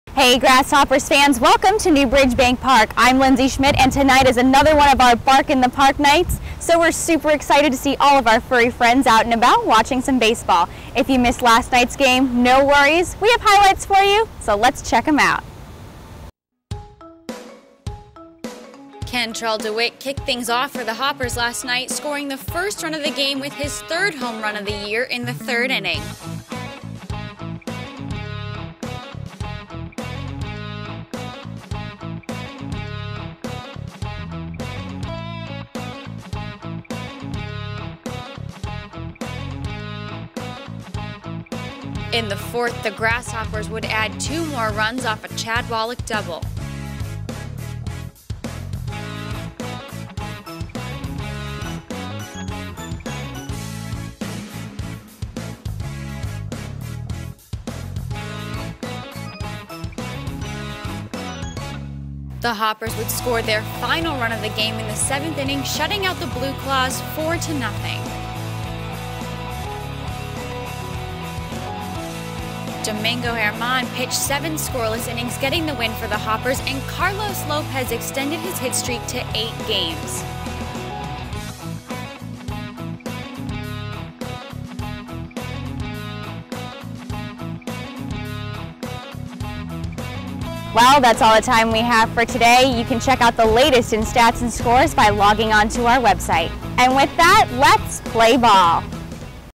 Hoppers Highlights, May 26, 2014
Highlights from the May 26, 2014 Greensboro Grasshoppers game against the Lakewood BlueClaws.